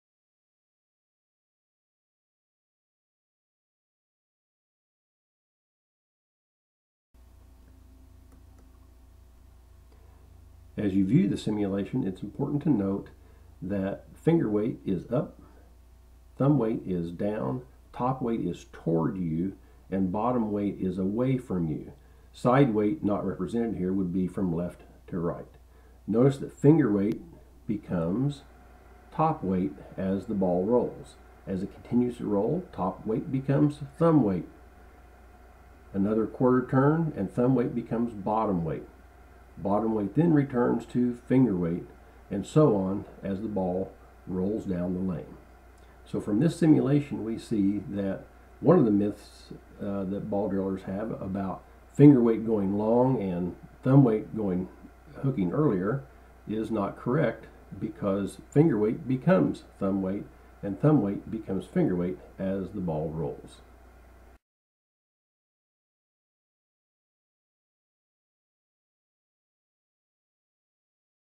Finger Weight Myth
Some bowling ball drillers believe the myth that finger weight makes a ball go "long" while thumb weight makes a ball "hook early".  This video shows a simulation that is designed to debunk that myth.